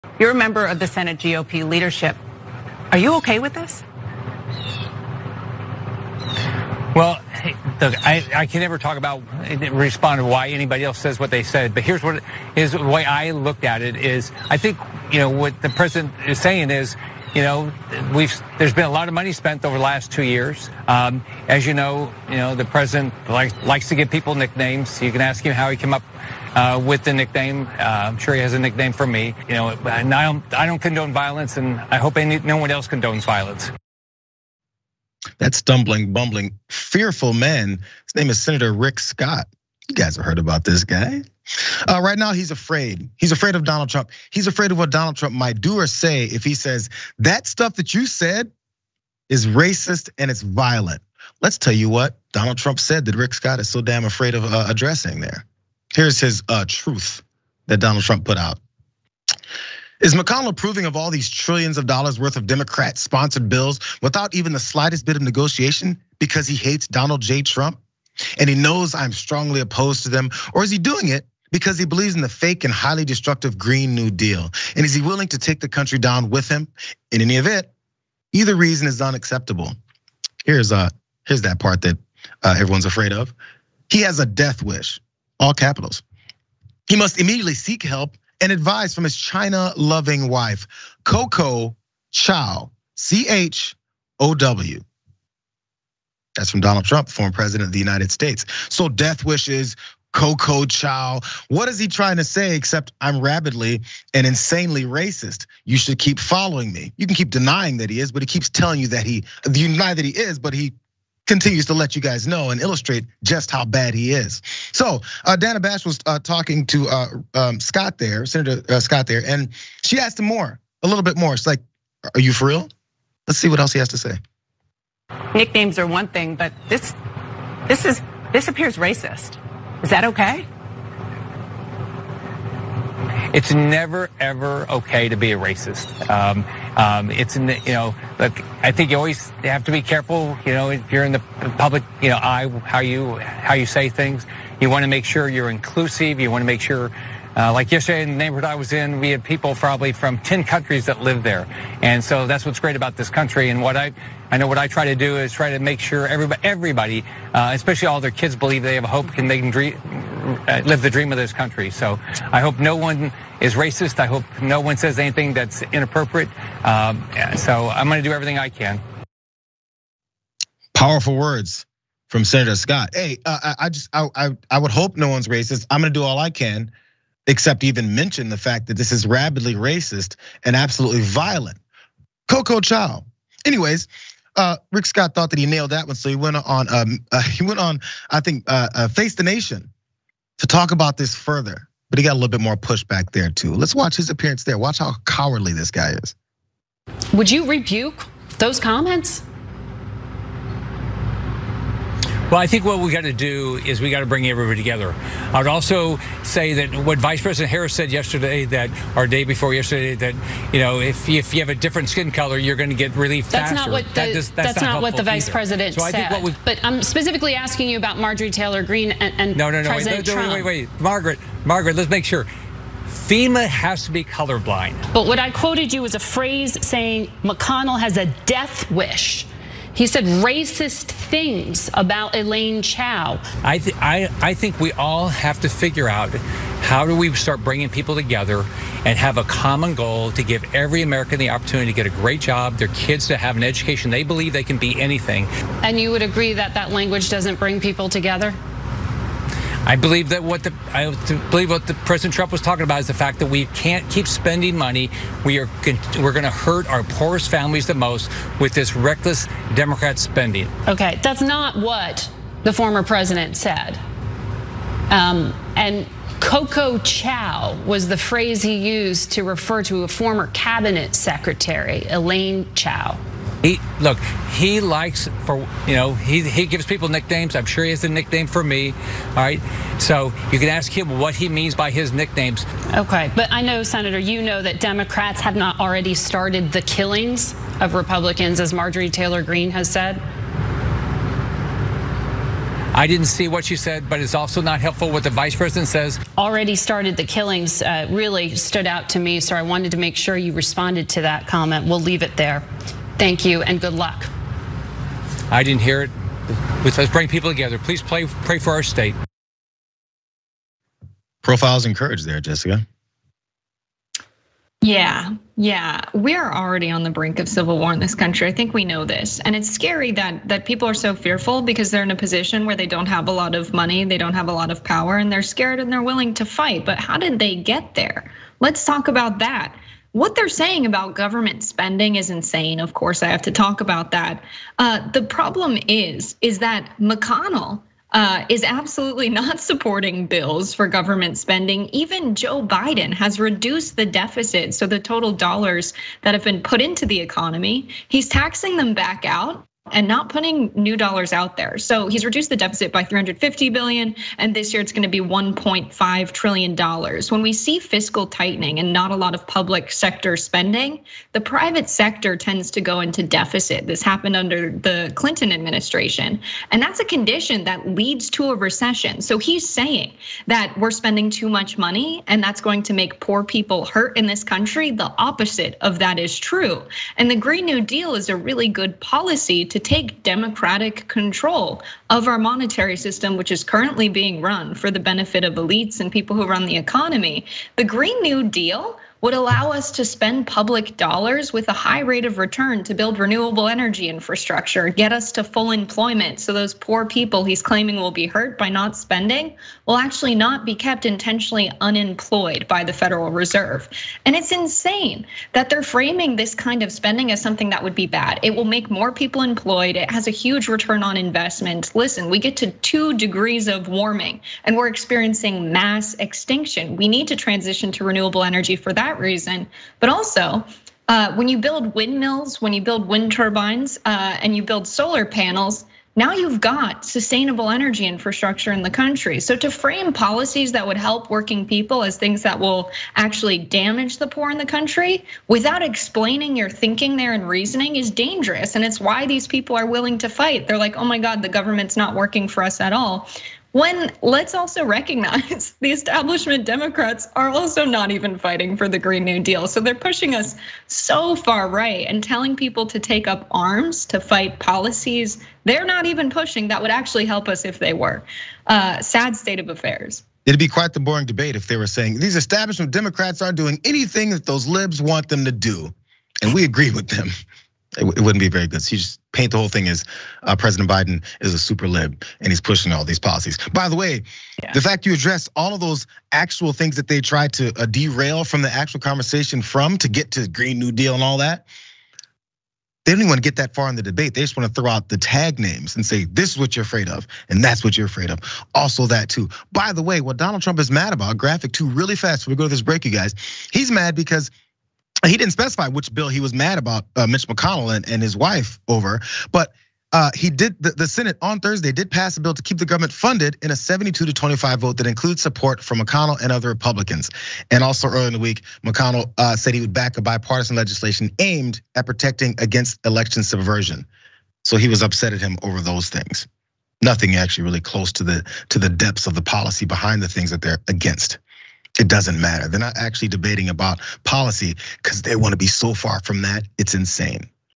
Rick Scott Wouldn't Dare Cross Daddy Trump
Senator Rick Scott is petrified of Donald Trump. He can barely piece together a sentence about his violence and racism, due to the fear he just might anger the petulant lord and savior of the republican party.Jayar Jackson and Jessica Burbank break it down on The Watchlist.

"Former President Donald Trump on Friday night directly ridiculed Senate Minority Leader Mitch McConnell, saying on his social media platform that the Kentucky Republican had a “death wish” for supporting “Democrat sponsored bills.”

Trump, in his Truth Social post, also mocked McConnell’s wife, Elaine Chao – who was born in Taiwan and served as Trump’s secretary of transportation – referring to her as McConnell’s “China loving wife, Coco Chow!”

Trump’s broadside at McConnell and mockery of Chao came hours after Congress approved and President Joe Biden signed a stopgap funding bill to avert a federal government shutdown. The bill cleared the Senate on a 72-25 vote Thursday and the House on a 230-201 vote Friday."

221003__WL03_Trump_Rages